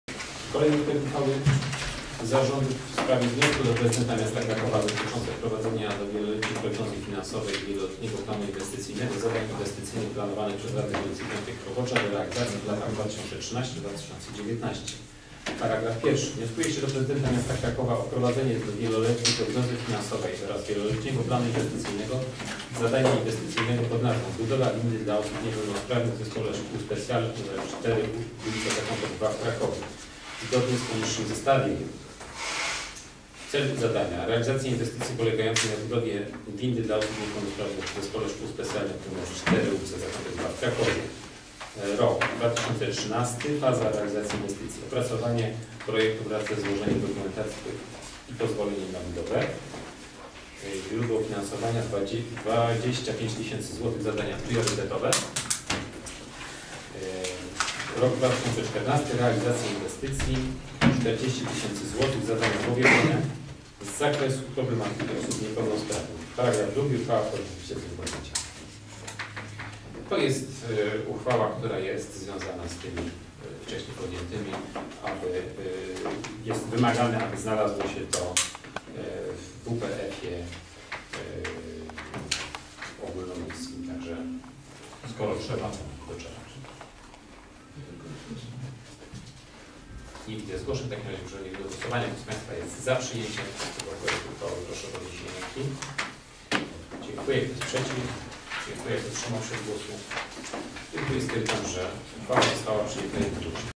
RD_V_17-09-2013_8 wniosek do Prezydenta Miasta Krakowa dot wprowadzenia zadań inwestycyjnych
Obrady XXXVI sesji Rady Dzielnicy V Krowodrza z dn. 17 września 2013 r.

Projekt uchwały Zarządu Dzielnicy V Krowodrza z dnia 9 września 2013 r. w sprawie wniosku do Prezydenta Miasta Krakowa dot. wprowadzenia do Wieloletniej Prognozy Finansowej i Wieloletniego Planu Inwestycyjnego, zadań inwestycyjnych planowanych przez Radę Dzielnicy V Krowodrza do realizacji w latach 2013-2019..